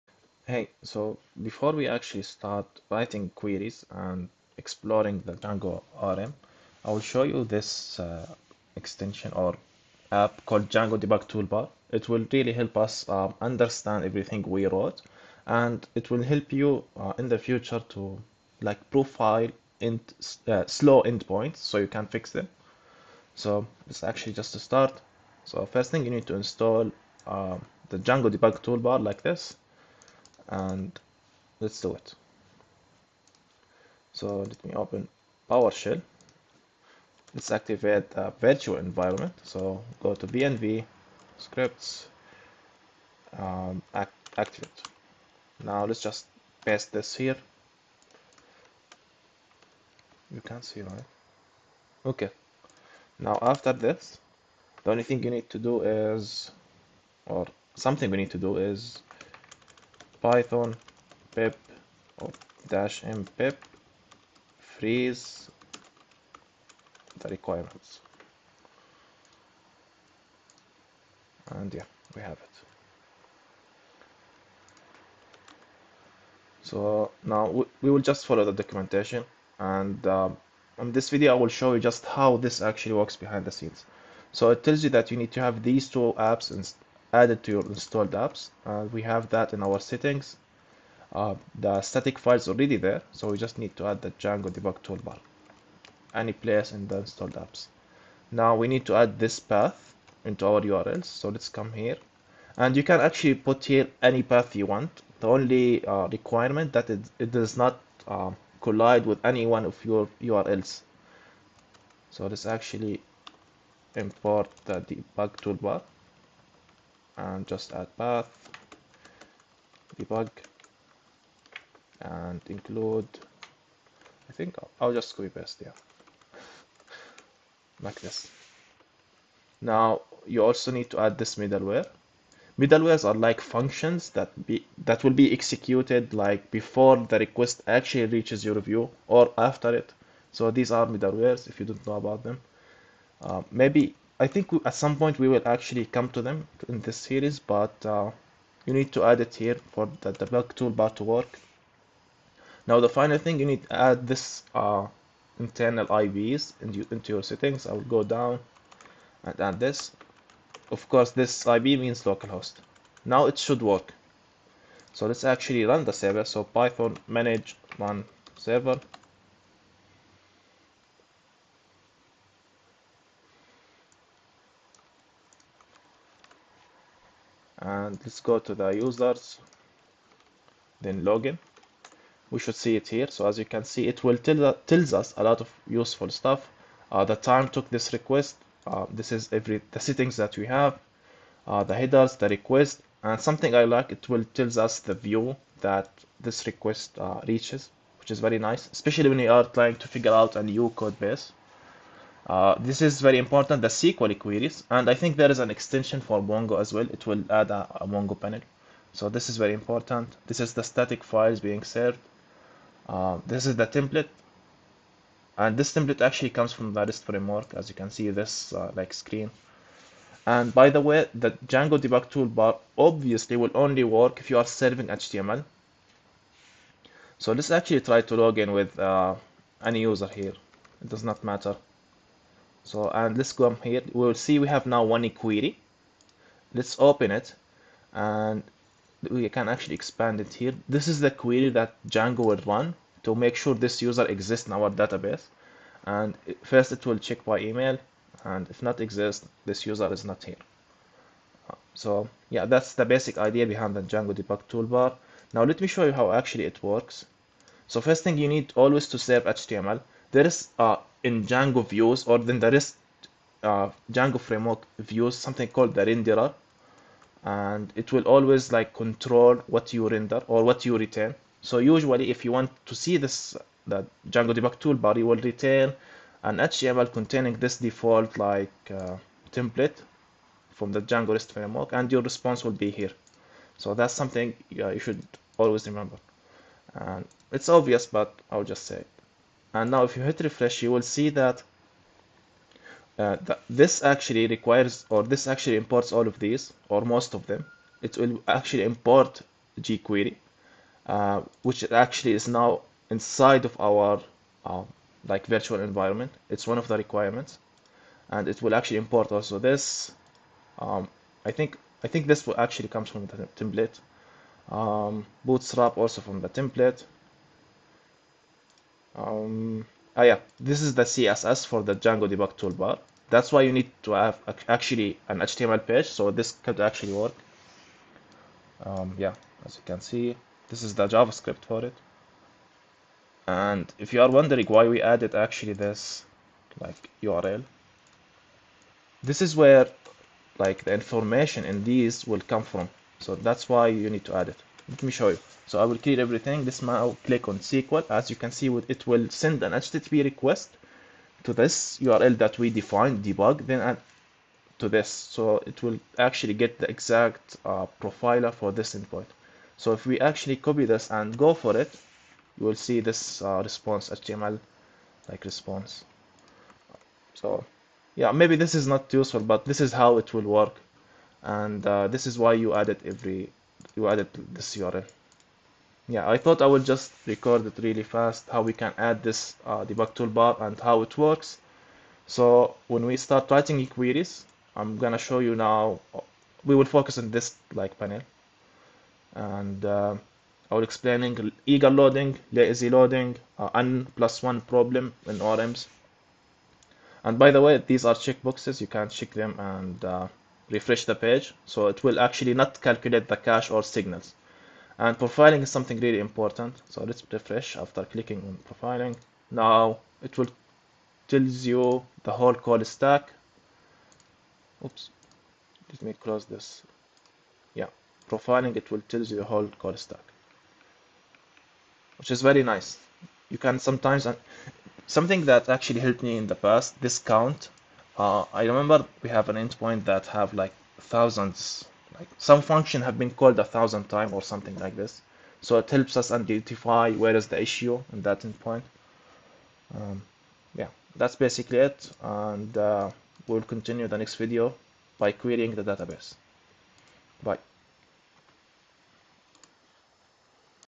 11 - Installing Django debug toolbar, small intro on how it works and why it's important
A full-stack series about Django, Django-rest-framework, react, redux/toolkit, and typescript